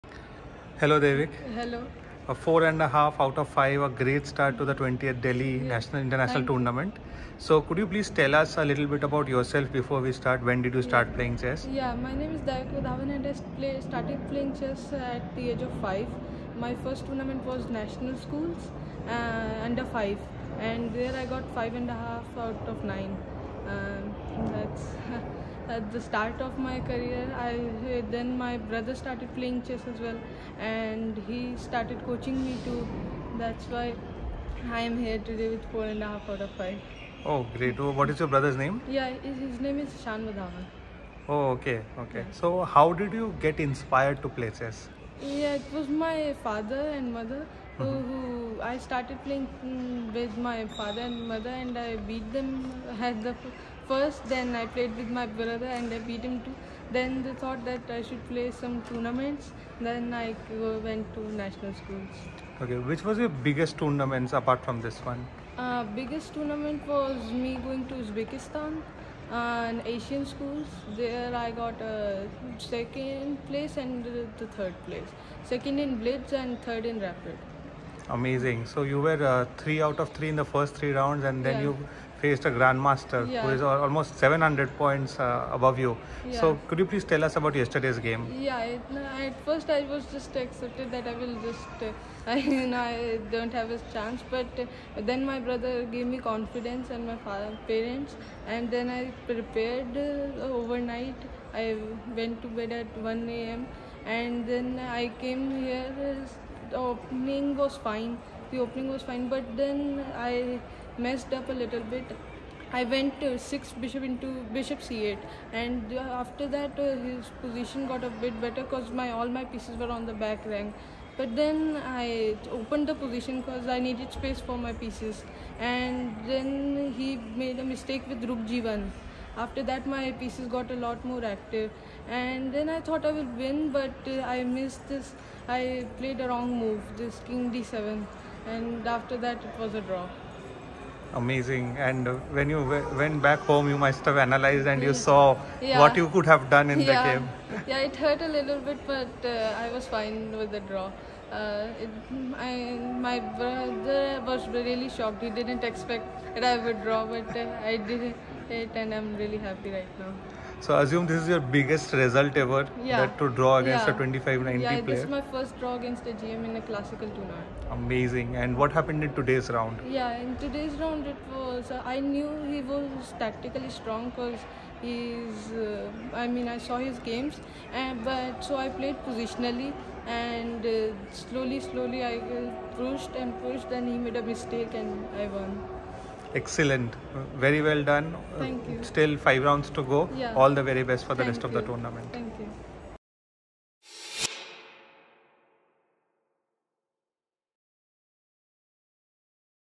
"I prepared till 1 a.m. before the game" - Daaevik Wadhawan on his spectacular start | Delhi GM 2023
Daaevik Wadhawan is currently rated 1886 and after playing only five rounds of the 20th Delhi International Grandmaster Open 2023, he has gained a massive 36.8 rating points. He talks about his early chess days and the contribution and support of his brother in his chess journey. In the video, he breaks down his game against Georgia ni.1, GM Levan Pantsulaia (2597).

Video:  Aditya Sur Roy / ChessBase India